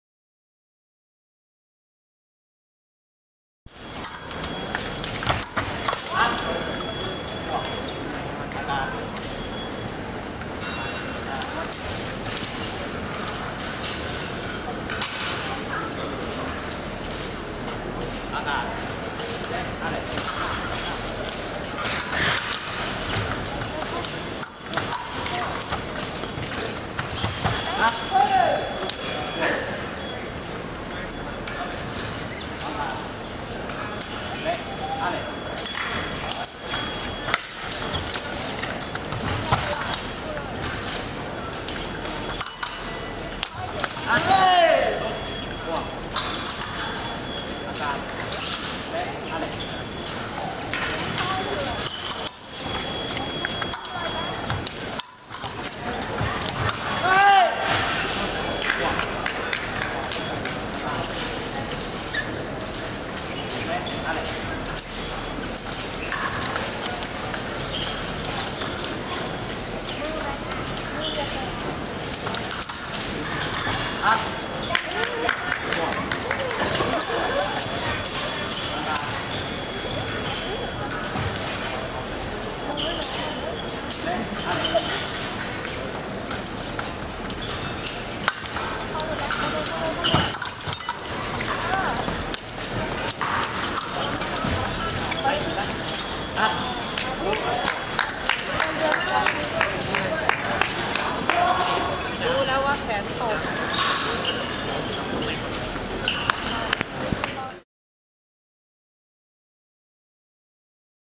Fencing Championship Thailand 2011 (2)
Th vs Th. Men's Individual Epee. 10th Navy Open Fencing Championship at Fashion Island Mall, July 2011, Bangkok, Thailand.